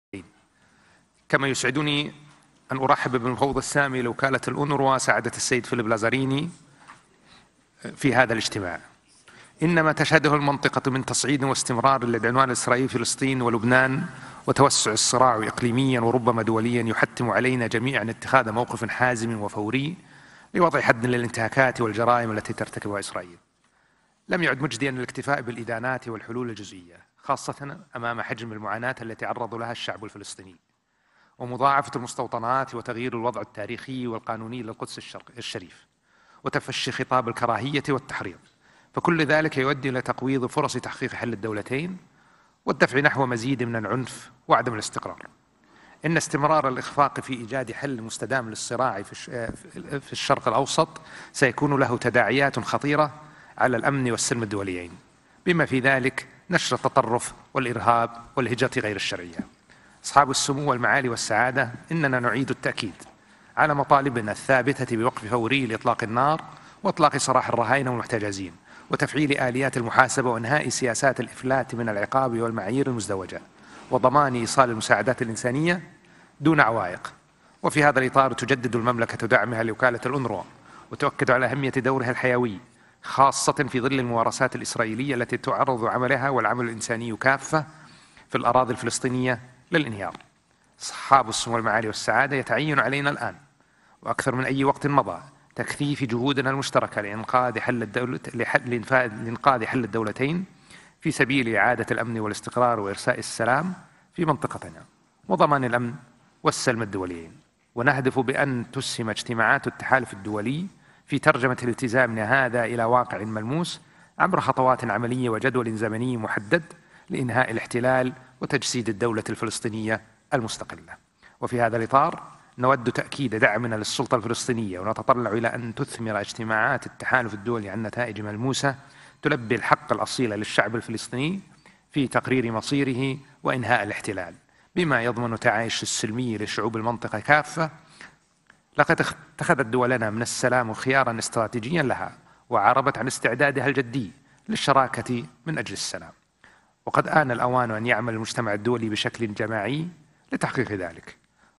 كلمة وزير الخارجية في الاجتماع الأول للتحالف الدولي لتنفيذ حل الدولتين
افتتح صاحب السمو الأمير فيصل بن فرحان بن عبدالله وزير الخارجية في العاصمة الرياض ، اليوم، الاجتماع الأول للتحالف الدولي لتنفيذ حل الدولتين الذي أعلنت عنه المملكة الشهر الماضي بالتعاون مع شركائها الدوليين.